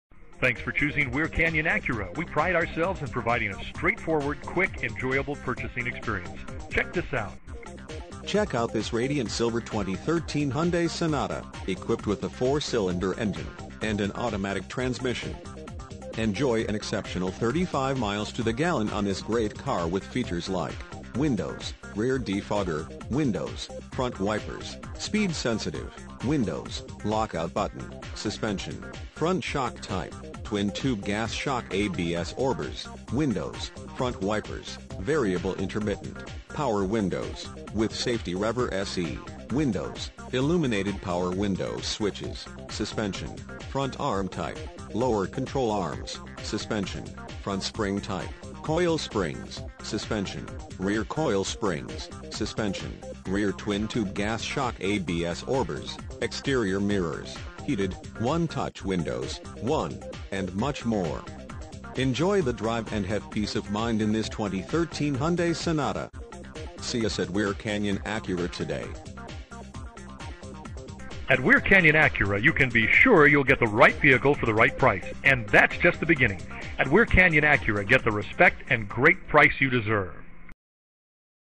2013 Hyundai Sonata Weir Canyon Acura Anaheim, CA 92807
Hyundai Sonata You will love this Radiant Silver Hyundai Sonata 4 Dr Sedan, equipped with a 4 Cylinder engine and an automatic transmission. Enjoy an exceptional 35 miles to the gallon on this great car with features like Windows, Rear Defogger, Windows, Front Wipers: Speed Sensitive, Windows, Lockout Button, Suspension, Front Shock Type: Twin-Tube Gas Shock Absorbers, Windows, Front Wipers: Variable Intermittent, Power Windows: With Safety Reverse, Windows, Illuminated Power Window Switches, Suspension, Front Arm Type: Lower Control Arms, Suspension, Front Spring Type: Coil Springs, Suspension, Rear Coil Springs, Suspension, Rear Twin-Tube Gas Shock Absorbers, Exterior Mirrors, Heated, One-Touch Windows: 1, Daytime Running Lights, Warnings And Reminders, Tire Pressure Warning, Seats, Front Seat Type: Bucket, Front Suspension Type: Macpherson Struts, Tail And Brake Lights, LED Rear Center Brakelight, Seatbelts, Seatbelt Pretensioners: Front, Tachometer, and much more. Enjoy the drive and have peace of mind in this Hyundai Sonata. See us at Weir Canyon Acura today!
MILES: 39,792
VIN:5NPEB4AC2DH637643

8323 E La Palma
Anaheim, California 92807